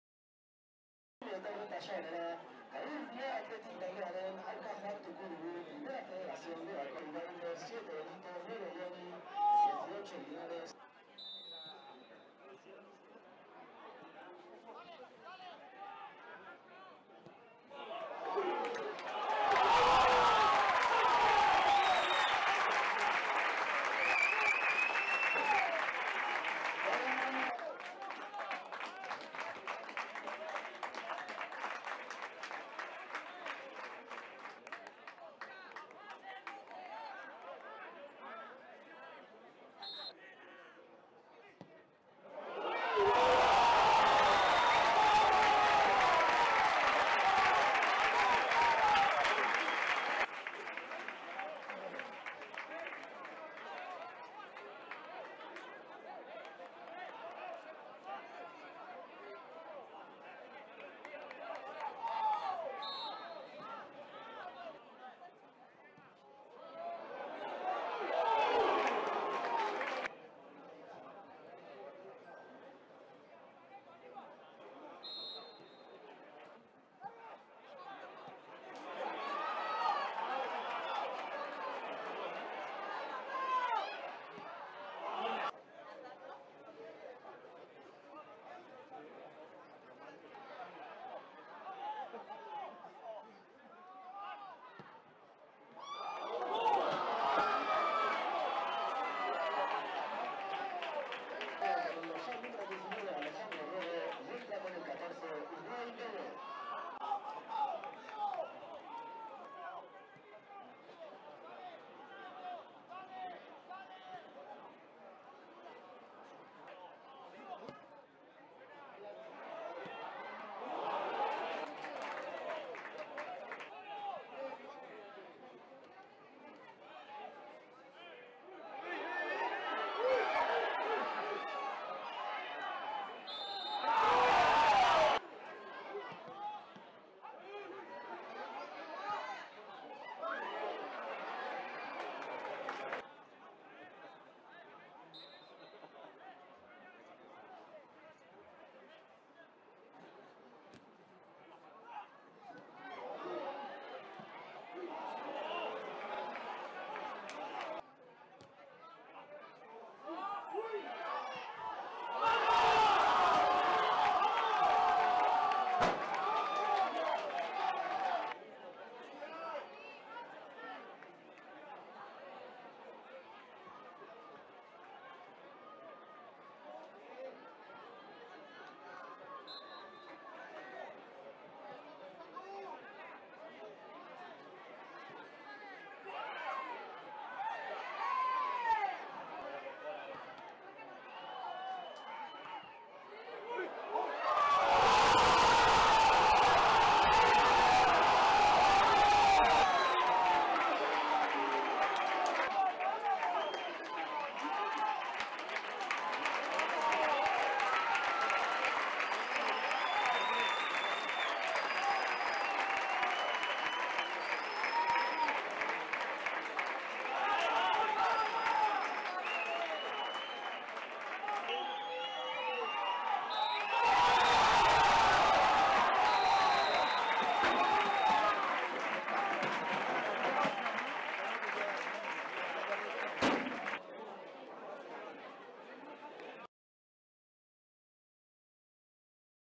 ⚽ Resumen I Osasuna Promesas 3-2 Bilbao Athletic I Laburpena I Primera Federación J13
Resumen de la derrota del Bilbao Athletic ante Osasuna Promesas en el partido correspondiente a la 13ª jornada de la temporada 2022-23 de la Primera Federación disputado en Tajonar.

Bilbao Athleticek Osasuna Promesasen aurka Taxoaren 2022-23 denboraldiko 13. jardunaldian jokatutako partidaren laburpena.